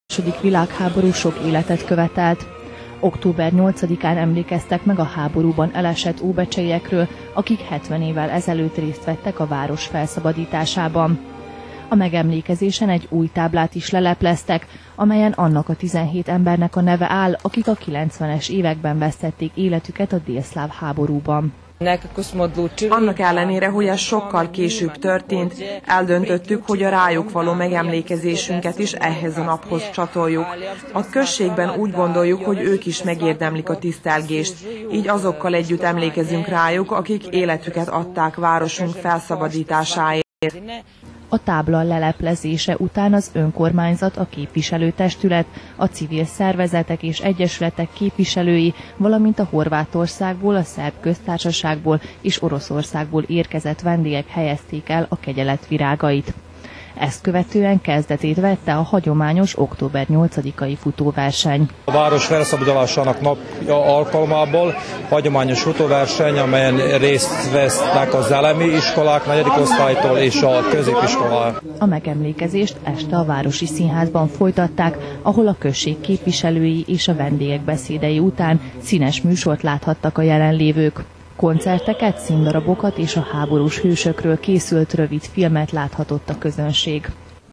Megemlékeztek a felszabadulásról
Óbecse felszabadításának 70. évfordulójáról emlékeztek meg és azokról az emberekről, akik életüket adtak városuk szabadságáért.